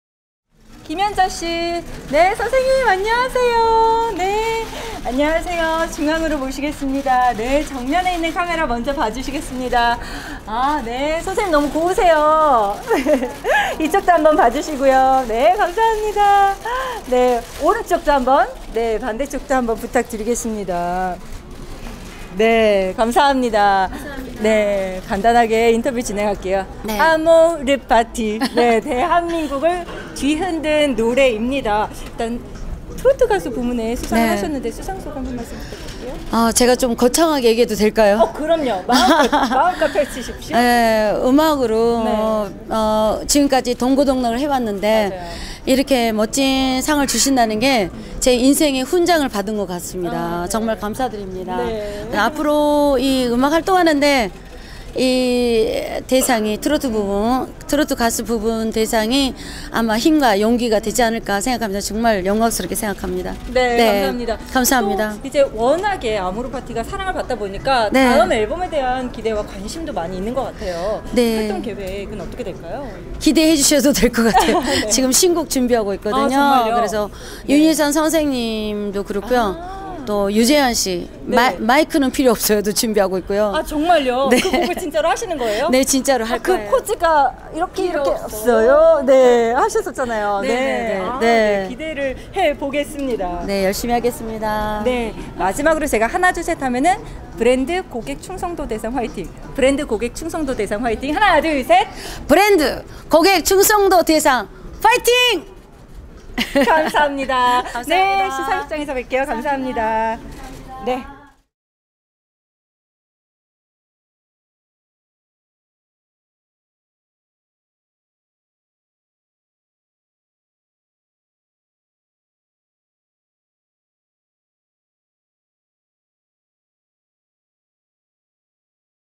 [BIG영상][4K] 김연자, 포토월을 지배하는 연자느님
17일 오후 서울 영등포구 여의도동에 위치한 콘래드 서울 파크 볼룸에서 '2019 브랜드 고객충성도 대상' 시상식에 앞서 포토월 행사가 열렸다.

이날 가수 김연자가 참석해 포토타임을 갖고 있다.

김연자, 브랜드 고객충성도 대상

4K, UHD, 빅데이터,빅데이터뉴스,연예,연예뉴스, 엔터테인먼트, 아이돌, 직캠, 레전드, Fancam, K-Pop